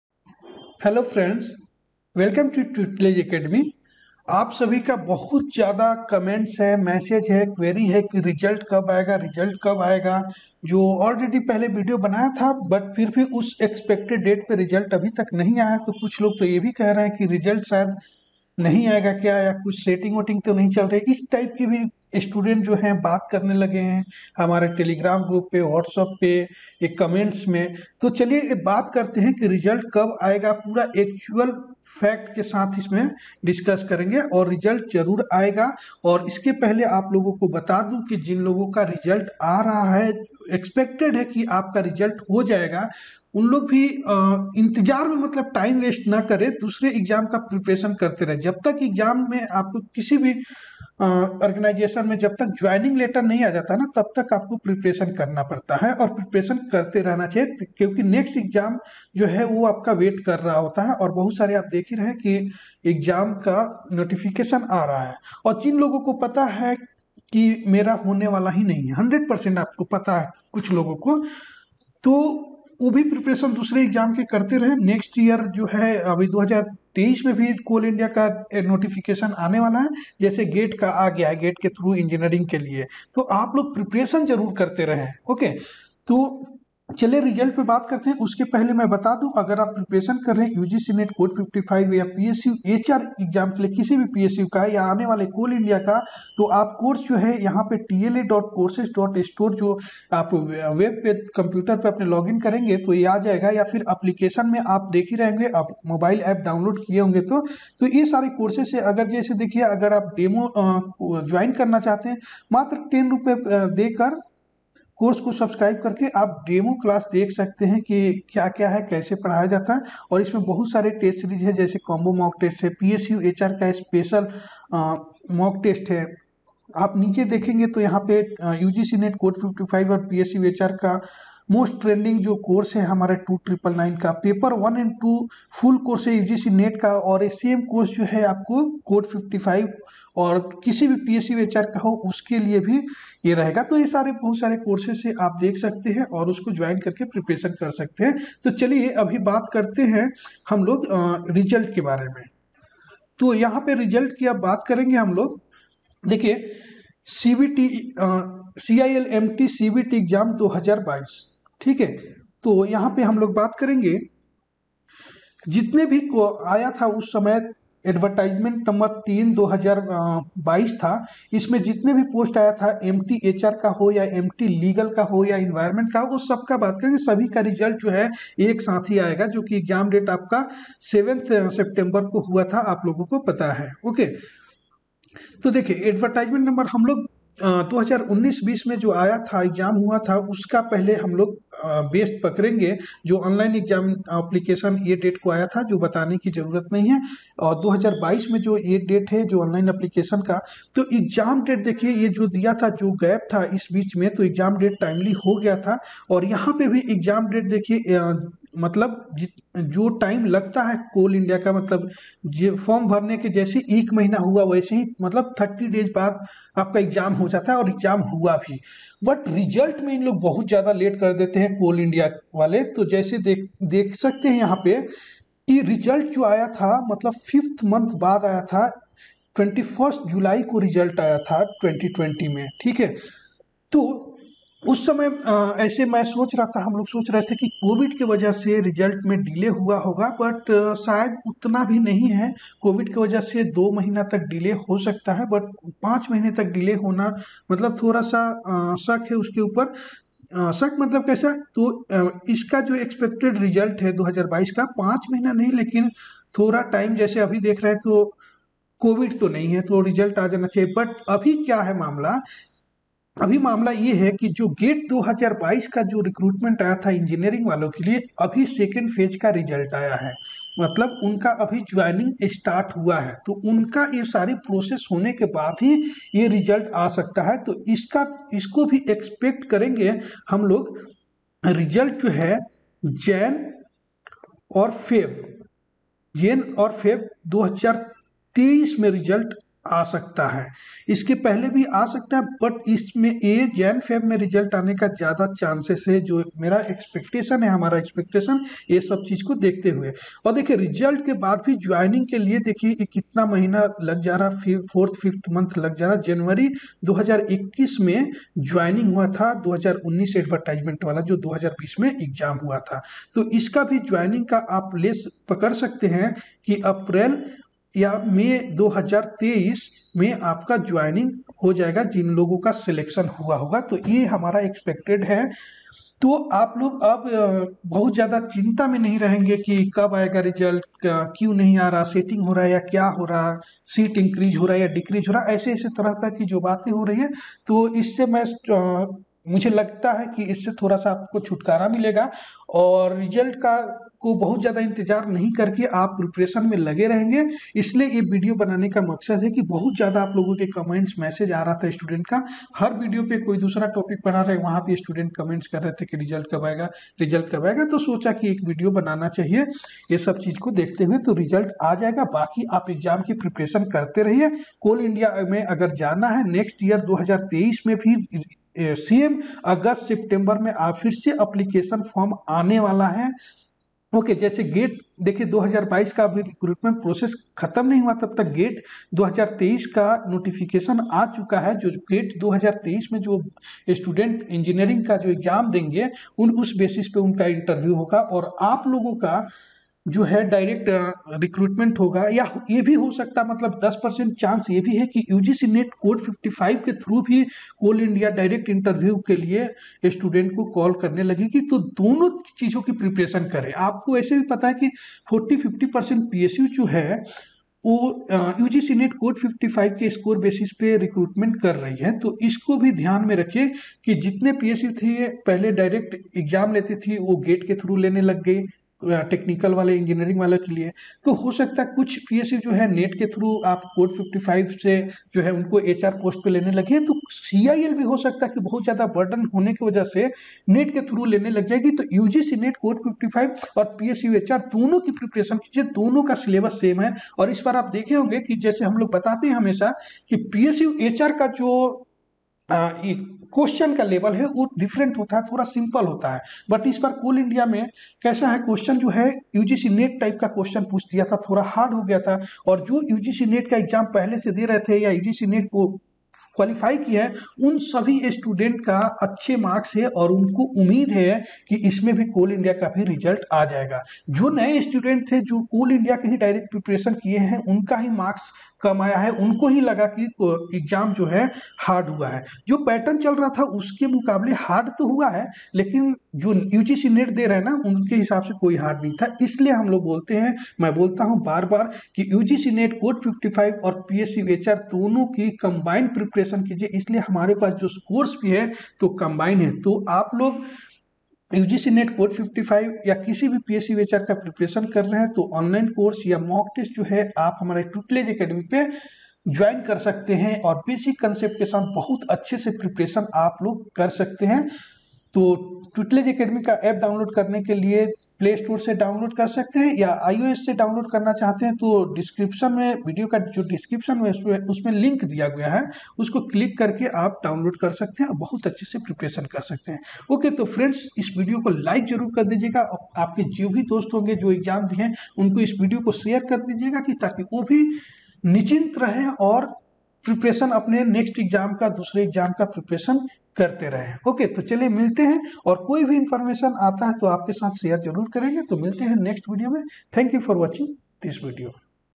CIL Expected Result 2022 MT-HR/Personnel and MT-Legal||Coal India Result इतना late क्यों हो रहा?
For Apple Store (ORG Code- TLA)

Join Best Online Course & Mock Test for CIL MT-HR/Personnel by our Expert & PSU HR, NET/JRF Qualified Facilities.

Coal India Limited CBT Exams 2022 MT Personnel & HR MT-Legal, Expected Result date by Nishikant Sir.